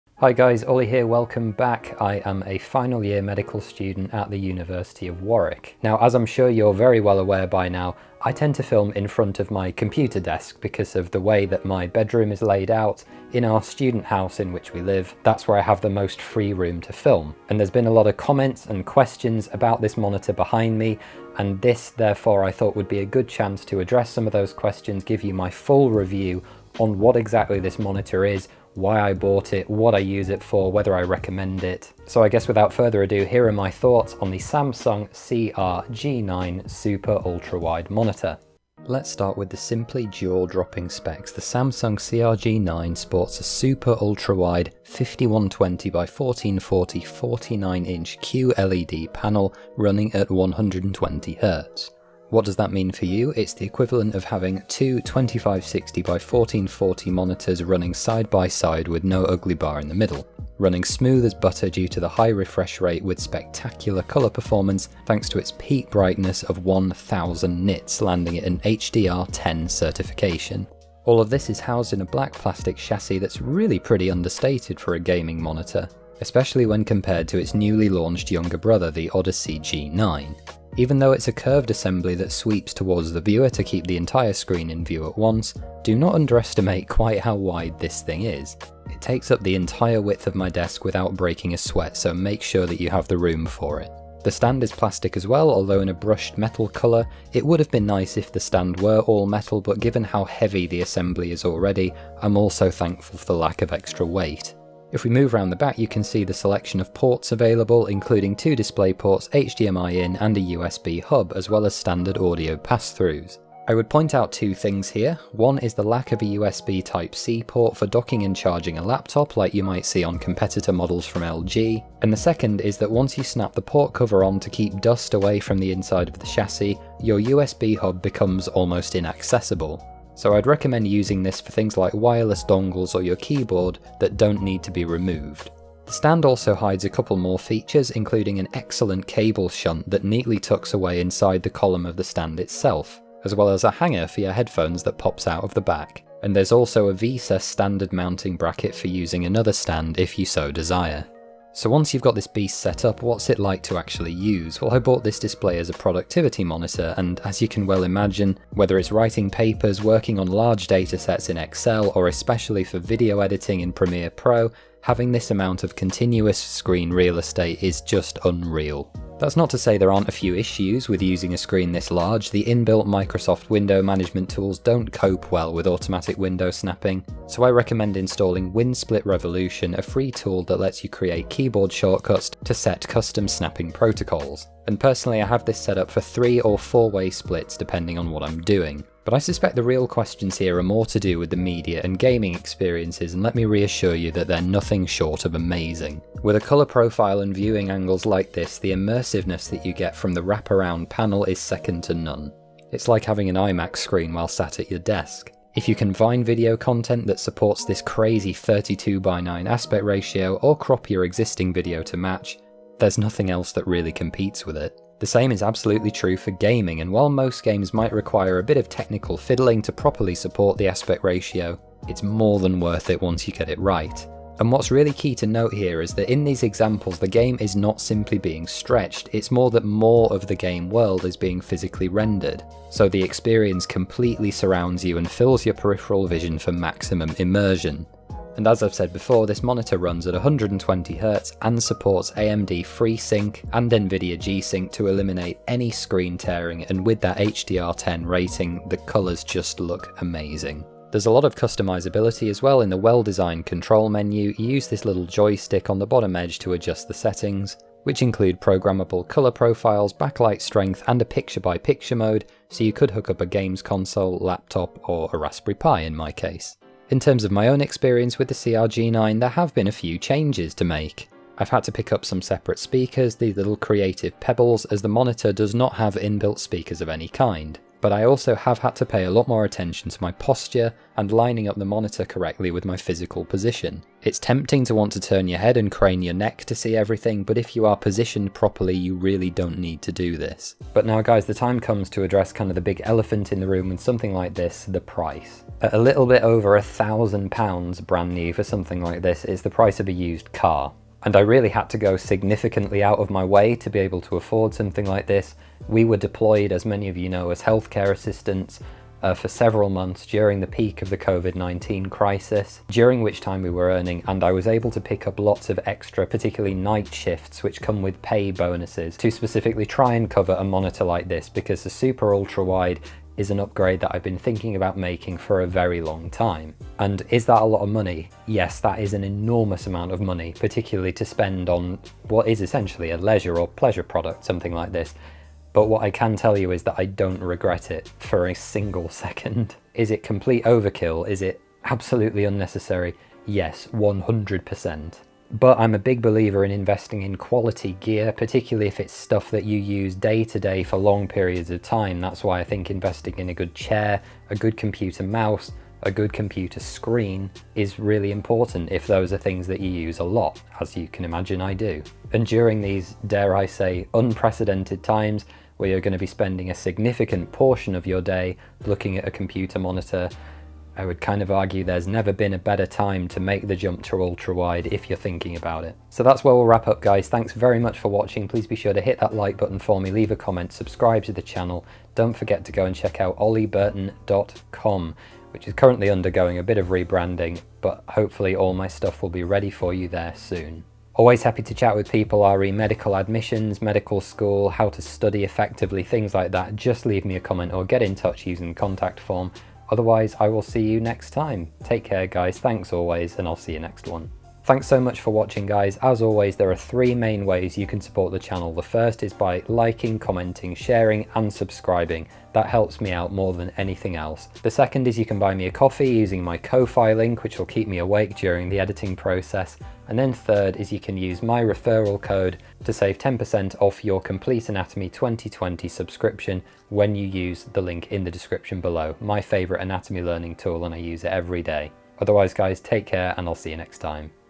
This GIANT MONITOR is INSANE | Samsung CRG9 49 Inch Ultrawide
Here at last, the review of the Super Ultrawide Samsung CRG9 PC monitor. It boasts a whopping resolution of 5120x1440 at a 120Hz refresh rate, and the multitasking/gaming experience is second to none on this bad boy.